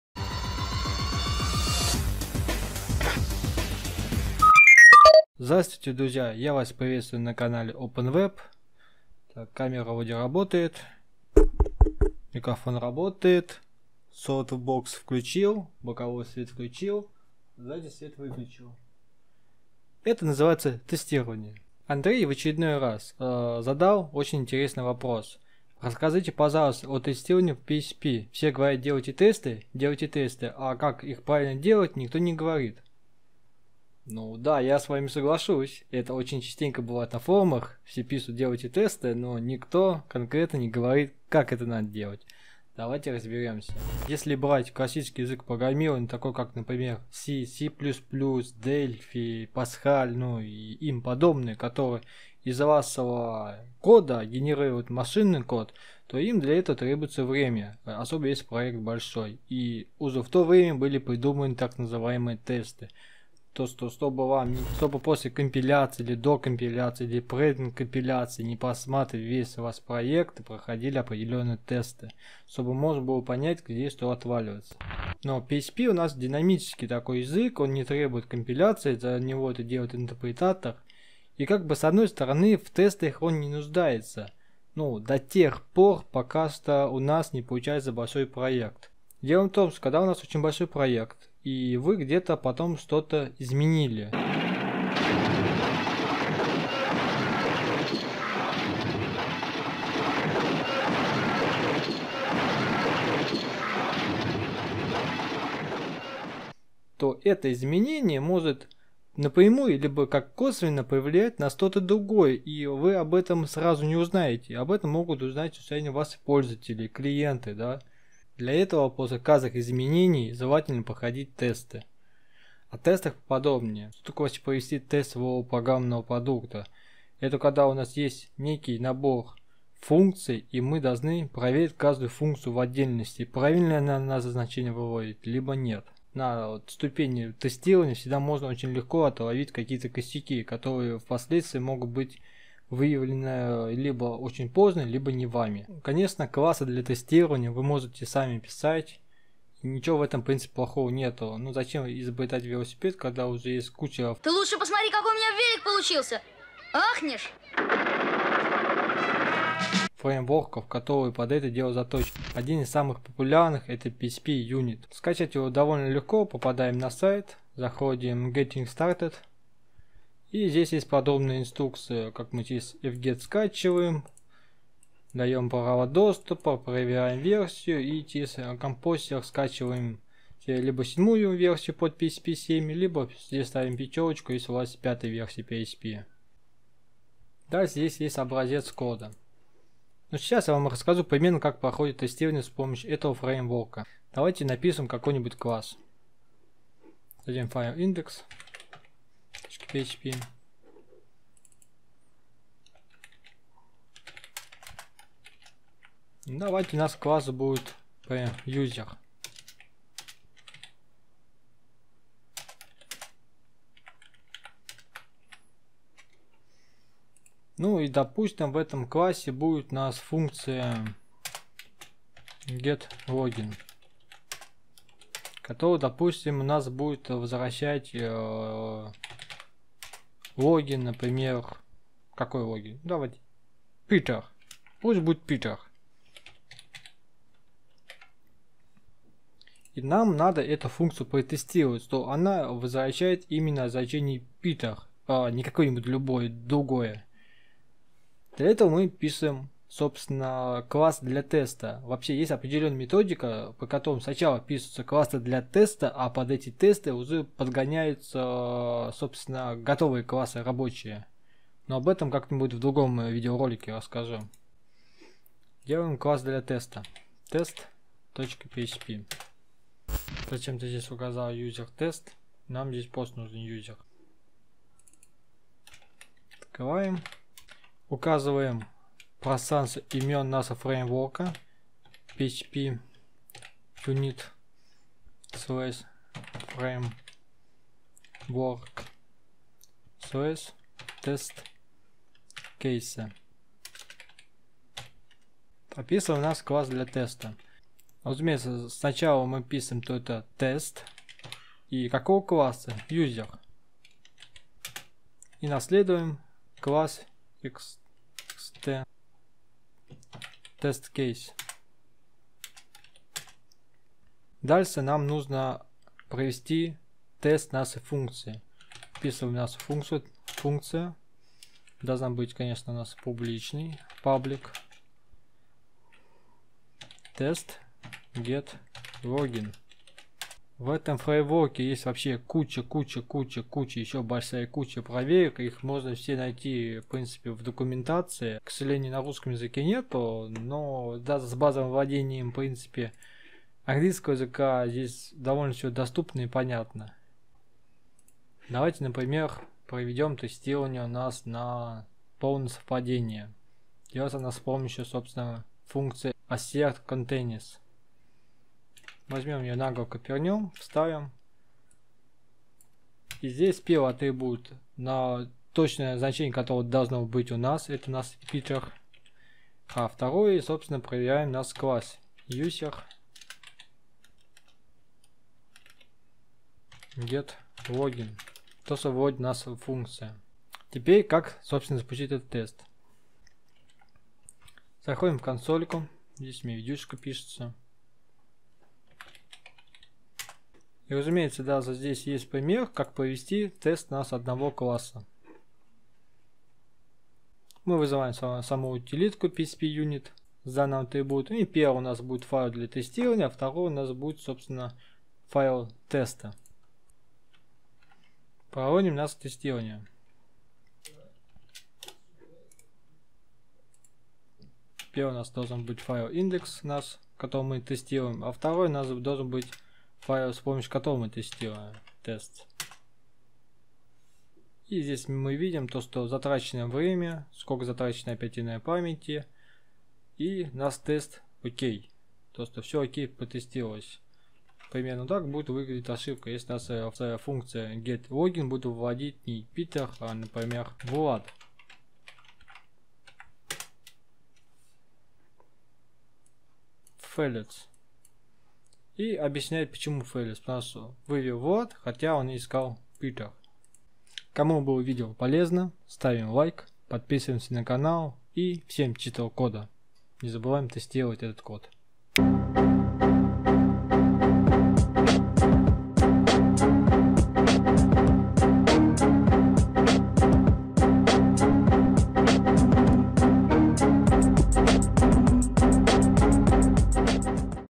PHPUnit тестирование кода php
Как протестировать свой код на php?
Вам поможет framework phpunit!

Задавайте свои вопросы в комментарий на интересные сделаю видео!